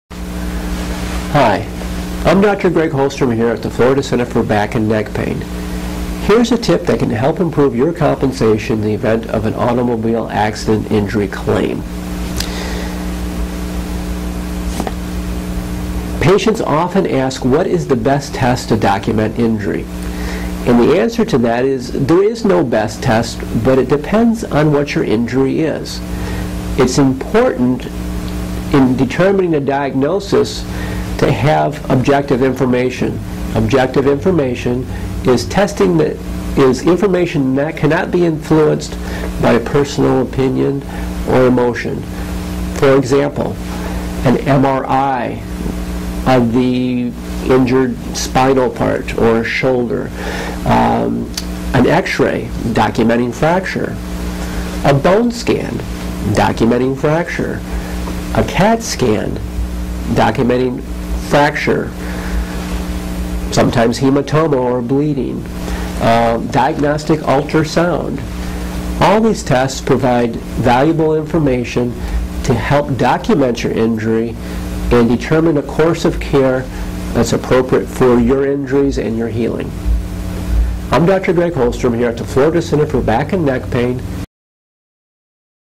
Testing Car Injury Compensation Largo FL
Car Accident Injury Claim and Car Injury Compensation explained by Chiropractor in Largo Florida, Dr. Greg Hollstrom.

Car Injury Therapy Contact Florida Center for Back and Neck Pain.

All Car Accident Injury Insurance Accepted.